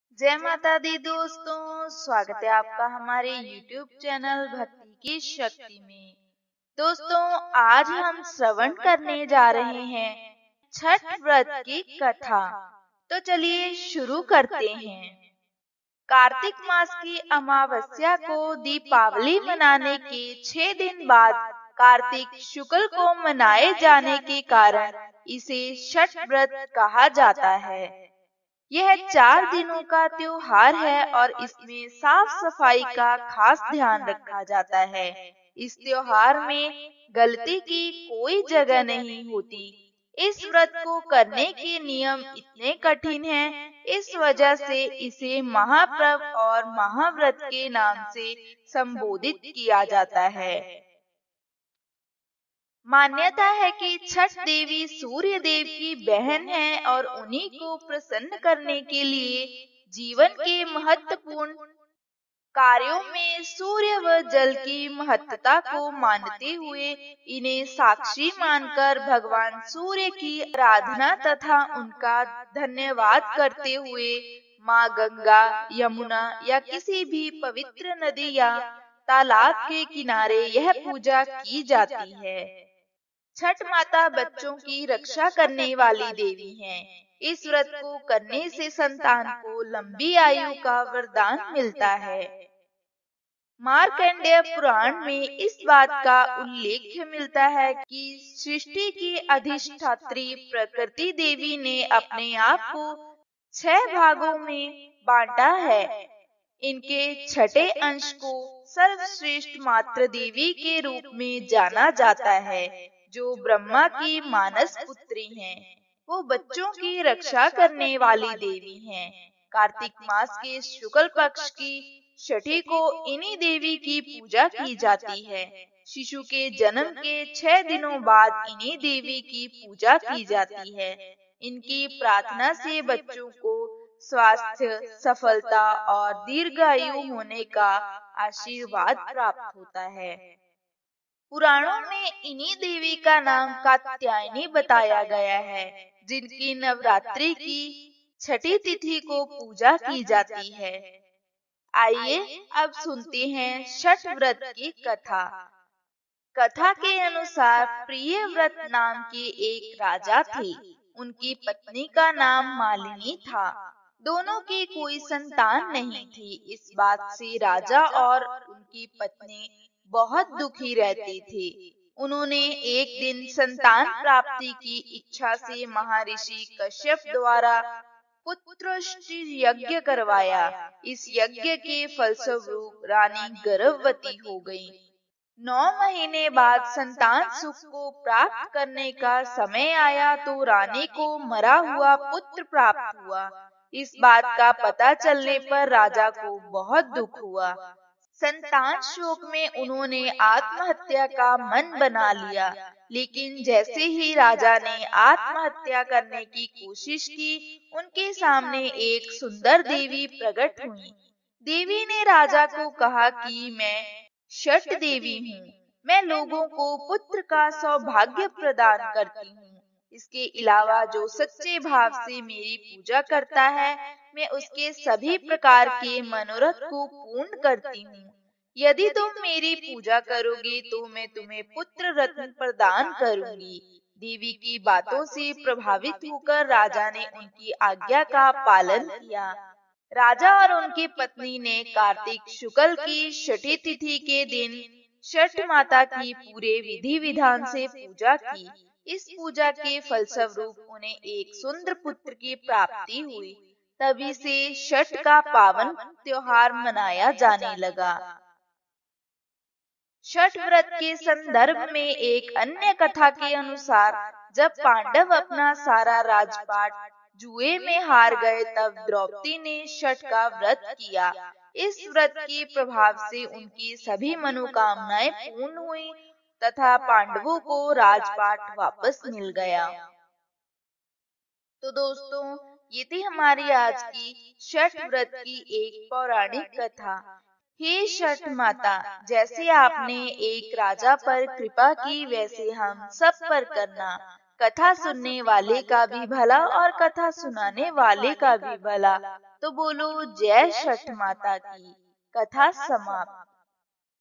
Chhath Puja Vrat Katha 2021 | छठ पूजा सम्पूर्ण व्रत कथा | छठ महापर्व पर ज़रूर सुनें छठी मैया की कथा
सबसे सस्ता सामान खरीदने के लिए इस लिंक पे क्लिक करें

छठ पर्व षष्ठी का अपभ्रंश है। कार्तिक मास की अमावस्या को दिवाली मनाने के 6 दिन बाद कार्तिक शुक्ल को मनाए जाने के कारण इसे छठ कहा जाता है।यह चार दिनों का त्योहार है और इसमें साफ-सफाई का खास ध्यान रखा जाता है। इस त्योहार में गलती की कोई जगह नहीं होती। इस व्रत को करने के नियम इतने कठिन हैं, इस वजह से इसे महापर्व और महाव्रत के नाम से संबोधित किया जाता है।मान्यता है कि छठ देवी सूर्य देव की बहन हैं और उन्हीं को प्रसन्न करने के लिए मां गंगा-यमुना या किसी भी पवित्र नदी या तालाब के किनारे यह पूजा की जाती है। षष्ठी मां यानी छठ माता बच्चों की रक्षा करने वाली देवी हैं। इस व्रत को करने से संतान को लंबी आयु का वरदान मिलता है।